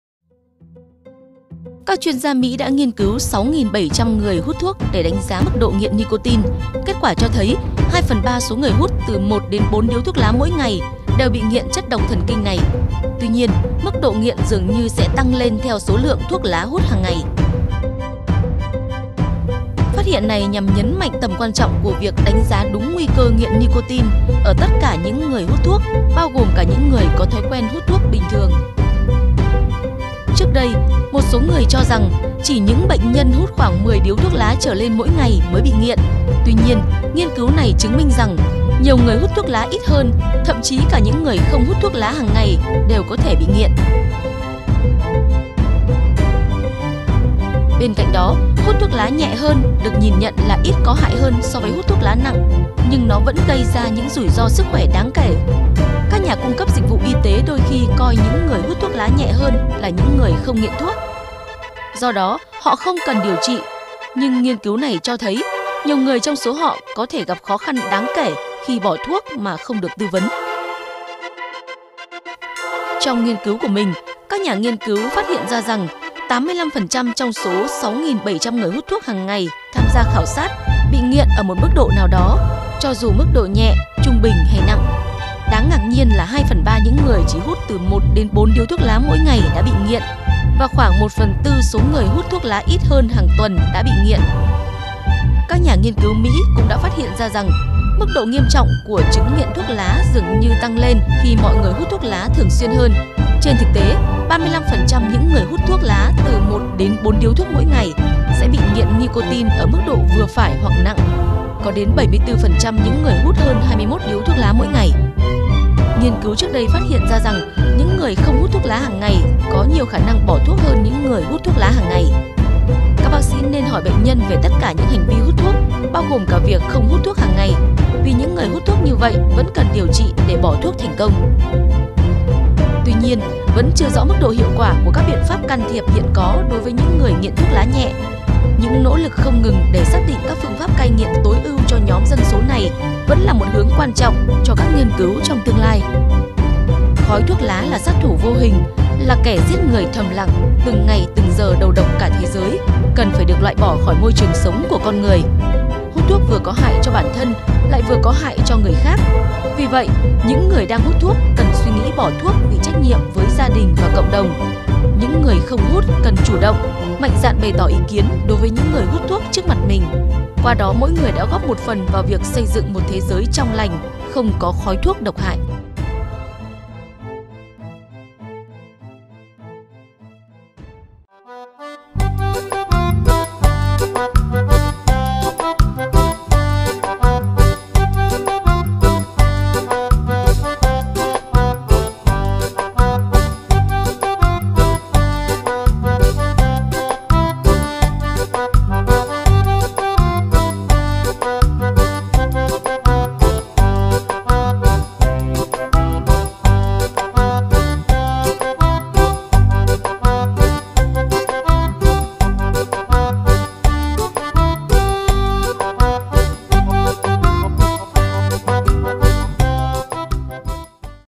Đừng chủ quan hút 1 điếu thuốc mỗi ngày cũng có thể gây nghiện
Nước súc miệng cai thuốc lá Thanh Nghị giúp người nghiện thuốc lá, thuốc lào cai thuốc hoàn toàn chỉ sau một liệu trình.